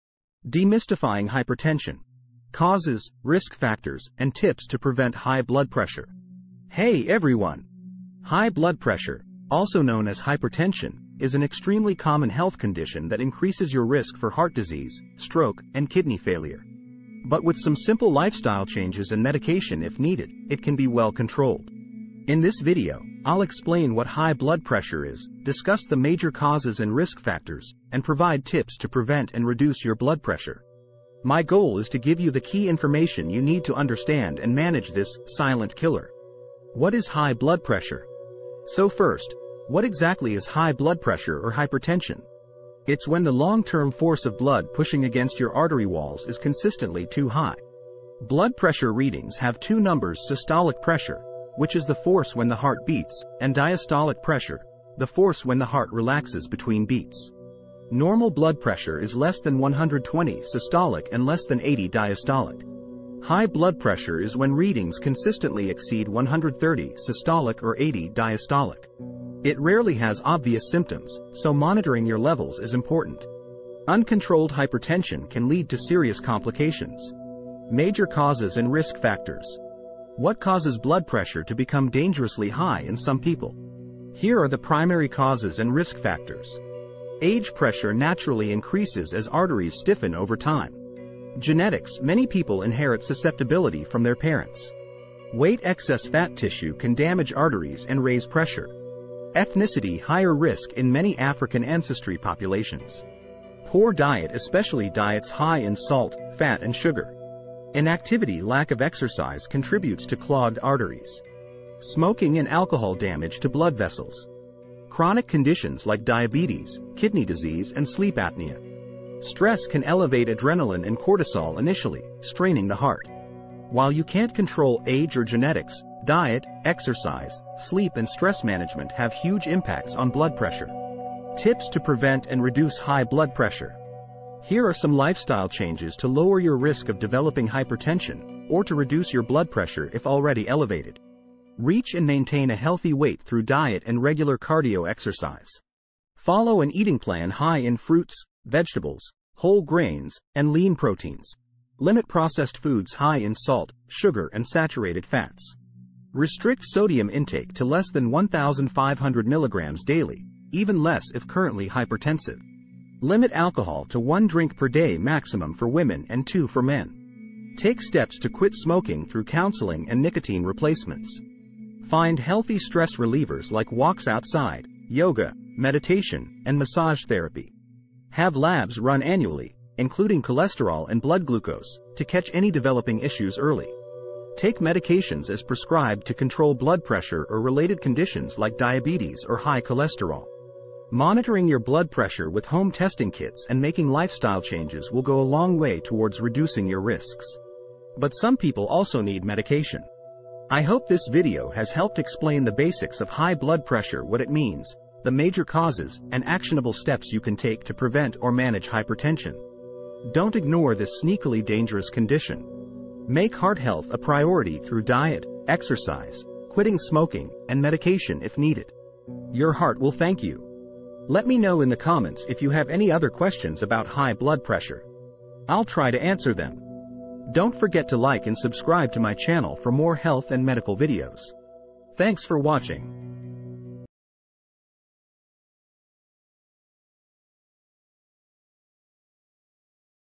Demystifying Hypertension: Causes, Risk Factors and Tips to Prevent High Blood Pressure.
In this insightful video, we delve into the world of hypertension, demystifying its causes, risk factors, and providing essential tips to prevent high blood pressure.